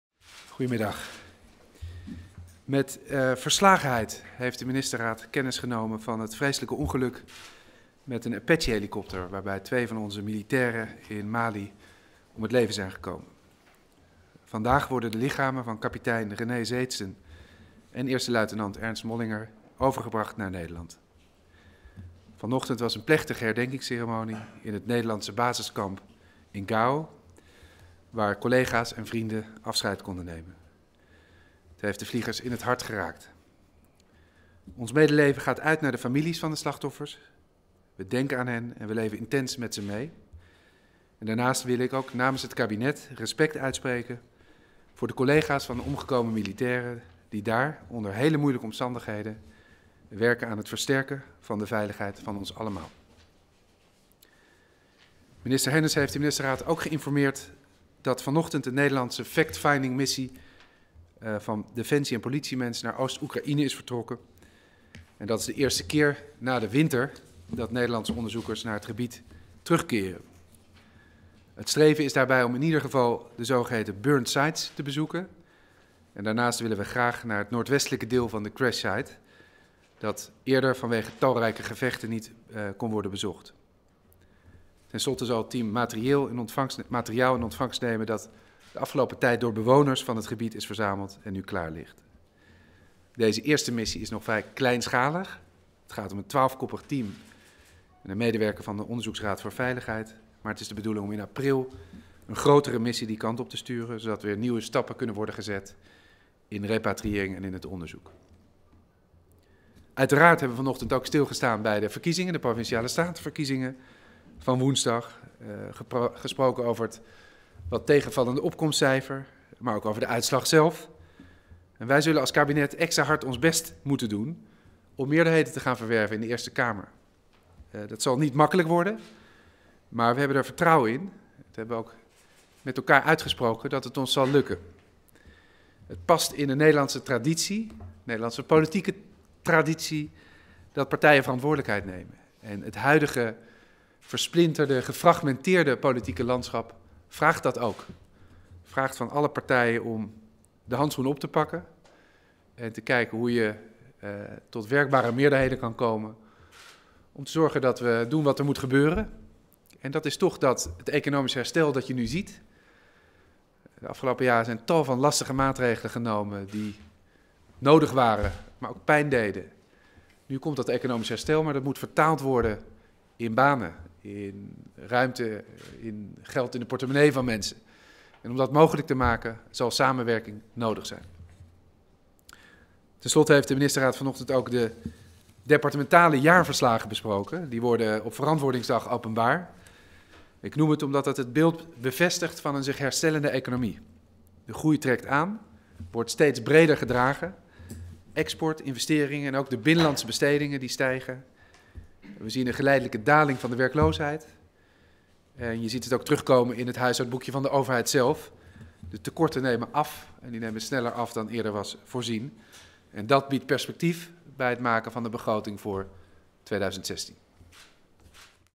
Vice-MP Asscher over ongeluk Mali en uitslag PS2015
DEN HAAG - Statement persconferentie viceminister-president Asscher na afloop van de ministerraad van 20 maart 2015. Asscher sprak onder andere over het verschrikkelijke ongeluk in Mali waarbij twee Nederlandse militairen om het leven zijn gekomen. Daarnaast stond hij stil bij de uitslag van de Provinciale-Statenverkiezingen 2015.